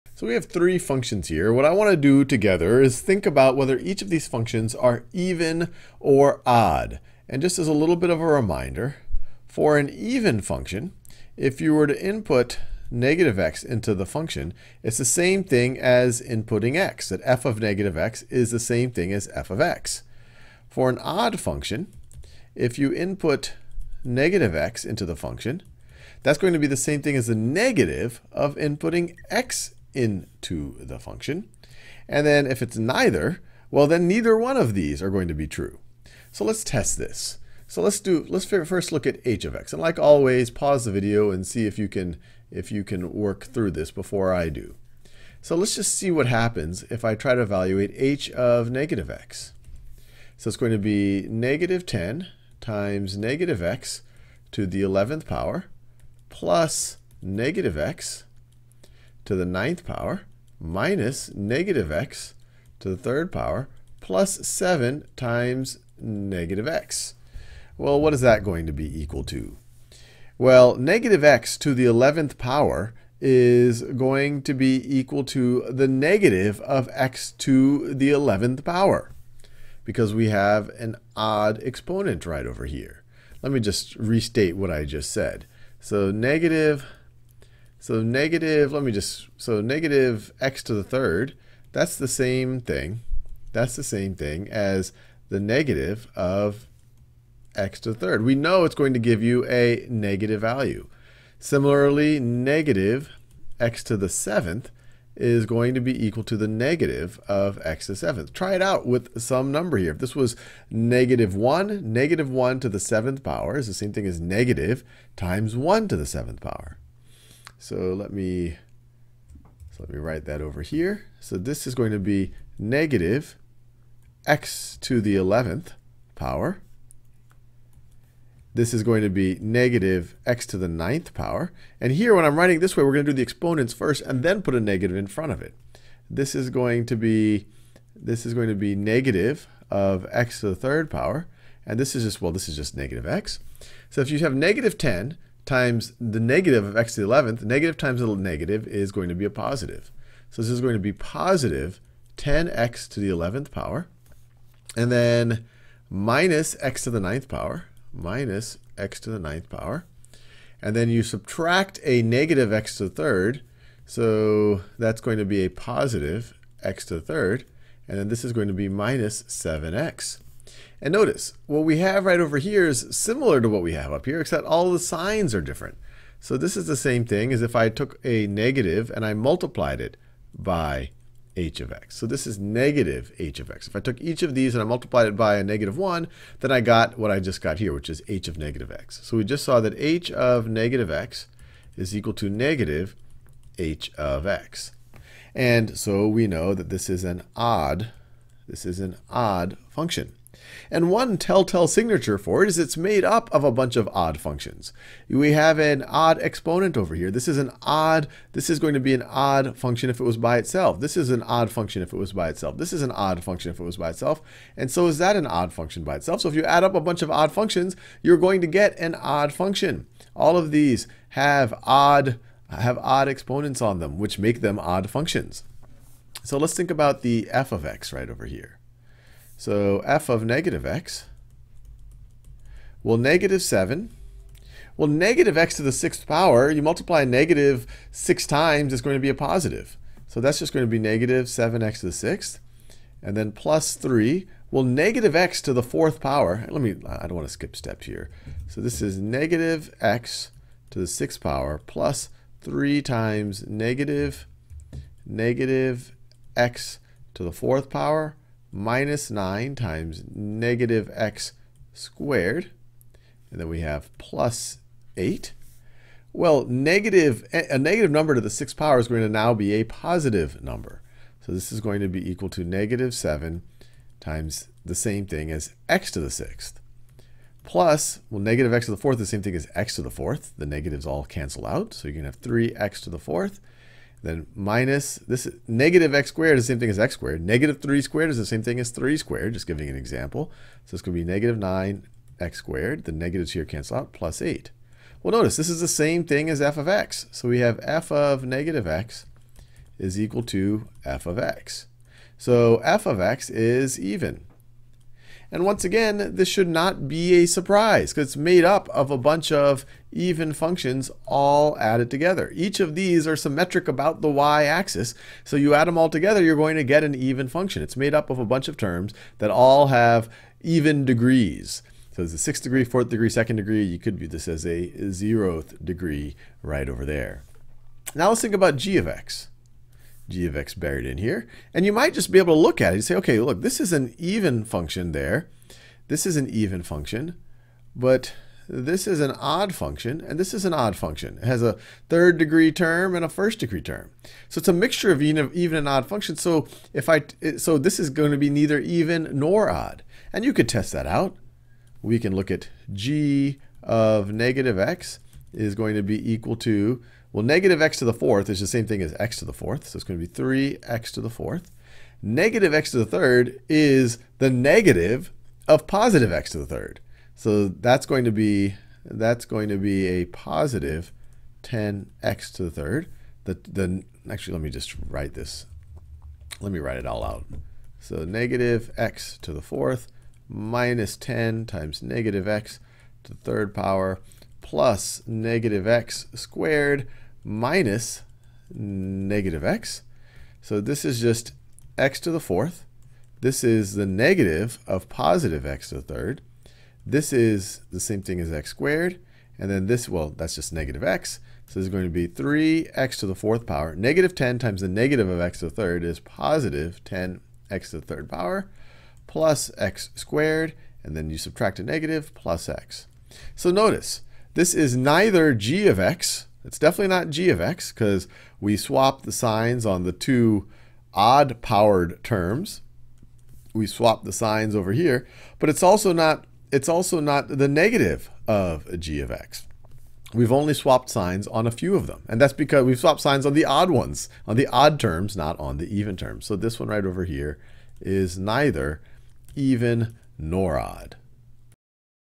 Even & odd polynomials | Mathematics III | High School Math | Khan Academy
Courses on Khan Academy are always 100% free. Start practicing—and saving your progress—now

Sal analyzes three different polynomials to see if they are even, odd, or neither.